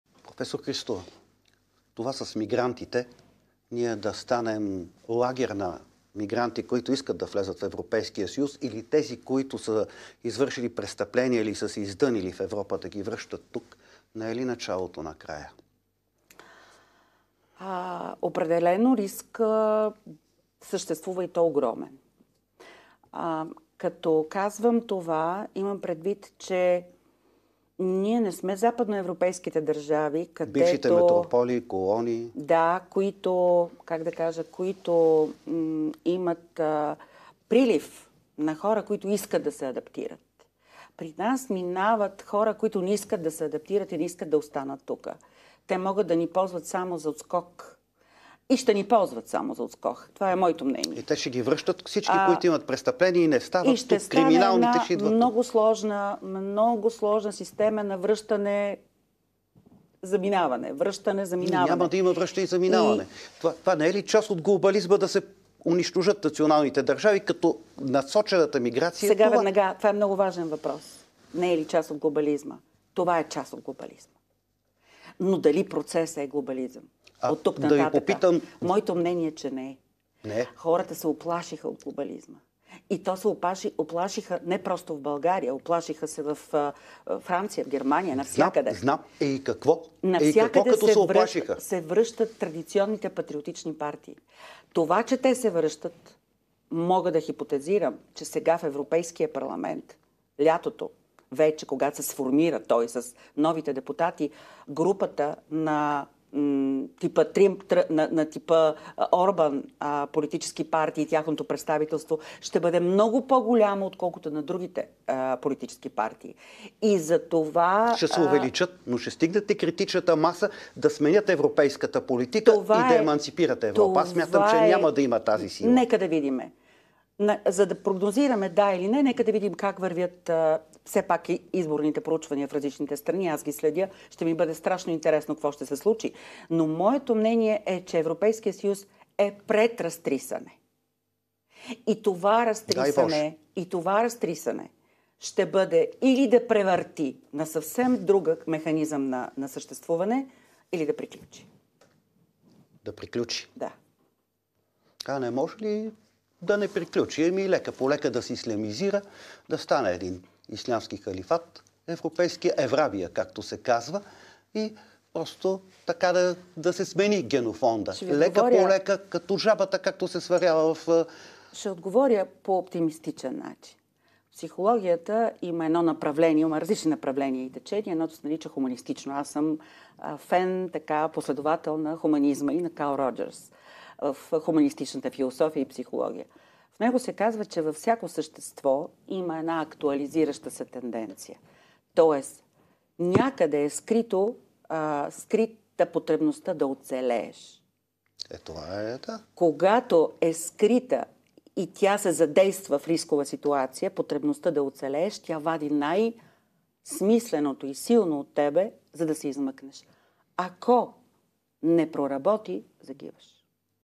Антоанета Христова за шансовете на Европа да оцелее
Антоанета Христова с интересен коментар, в който посочва, че единствено автентичните консервативни европейски партии имат шанс да променят системата в ЕС. В българия техен представител е възраждане на